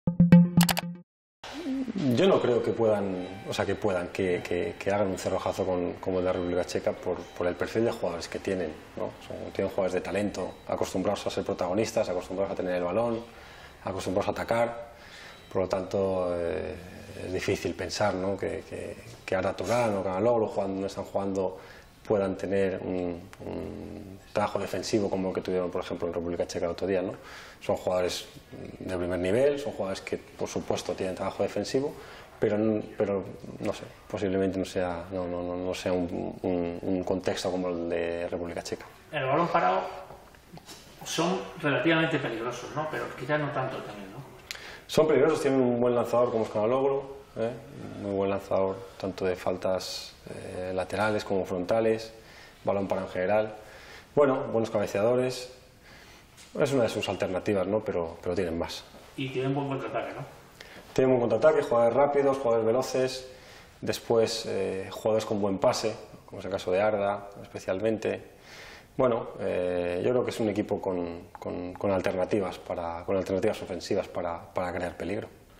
Albert Celades analiza el juego de Turquía, próximo rival de España
Madrid, 16 jun (EFE).- El entrenador de la selección española de fútbol sub21, Albert Celades, considera que Turquía, próximo rival de España en la Eurocopa de Francia, no hará, "por el perfil de jugadores que tiene", un "cerrojado".

Celades, quie se encarga de facilitar al entrenador y jugadores de la selección información sobre sus rivales, asegura que "es difícil pensar" que jugadores como Arda Turan "puedan tener un trabajo dwefensivo como tuvieron los de la República Checa".

IMÁGENES CEDIDAS POR ATM